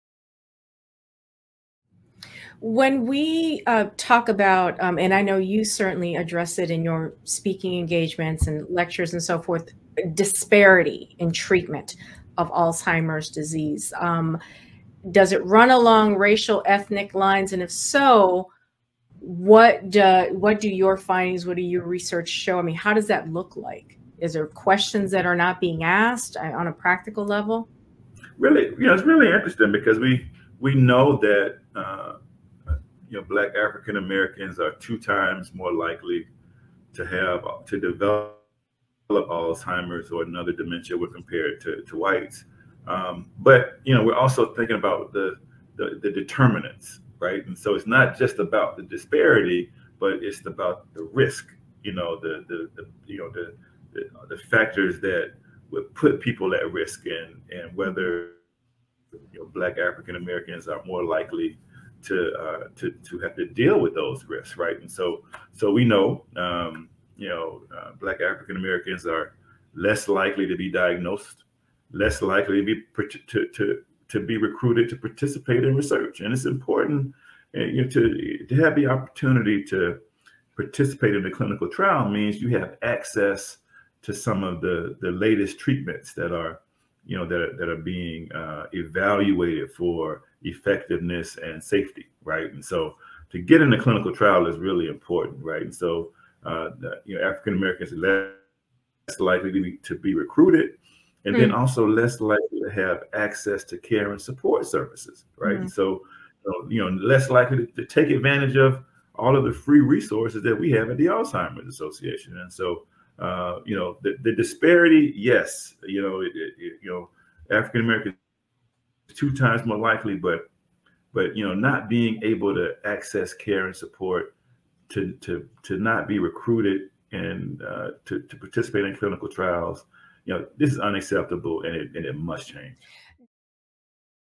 Ep. 4: Podcast Clip: Does the Disparity in Alzheimer's Treatment Run Along Racial/Ethnic Lines?
This is an excerpt from an interview with Dr. Carl V. Hill, Chief Diversity, Equity & Inclusion Officer for the Alzheimer's Association on the Wills, Women & Wealth Podcast.
Listen to Dr. Hill's comments on the disparity of treatment of Alzheimer's disease and whether it runs along racial or ethnic lines.